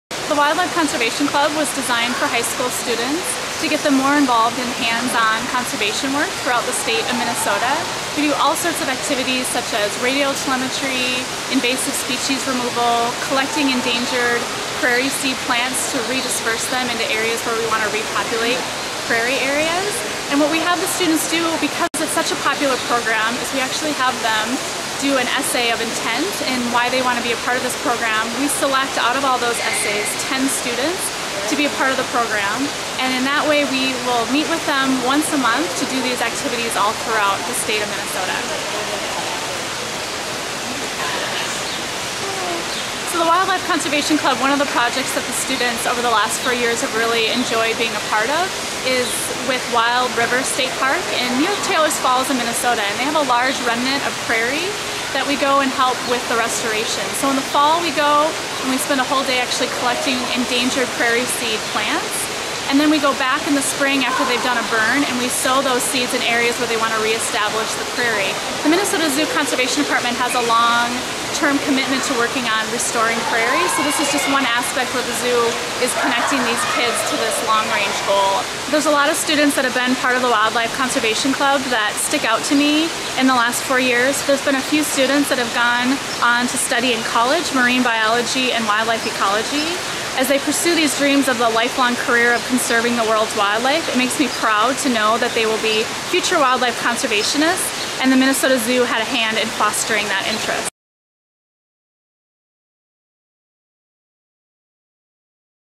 GTMD Youth Video
The Minnesota Zoo is committed to educating and engaging today's youth on the importance of wildlife and habitat conservation through various programs including the Wildlife Conservation Club. In participating with these programs, youth are able to get hands-on experience in working with the many conservation programs around the world the Minnesota Zoo is committed to.